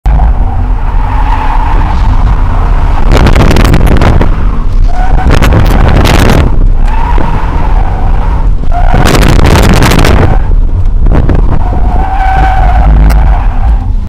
Open diff manji drift
BMW E87, pilot sport 4 in the front axle, crap tires in the rear, some water in the ground.